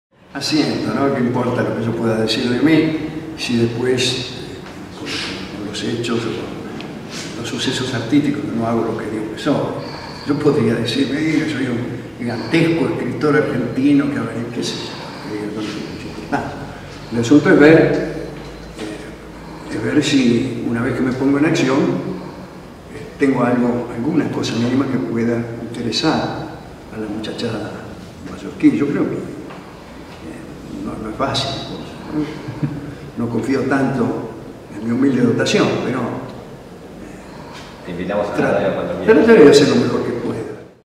Entrevista a Alejandro Dolina 5. Palma. Fest jaja
Entrevista a Alejandro Dolina que vino a Palma a presentar "La venganza será terrible" acompañado de su inseparable compañero Patricio Barton y un invitado muy especial, Luis Piedrahita; de la mano de Fes Jaja, Festival del humor, nos dejaron robarle diez minutos de este gran personaje y gran persona que muy amablemente nos atendió con gusto y buen humor.

Agradecer a Jorge Guerreño por la entrevista y a la gente se Six producciones por el gran trabajo visual.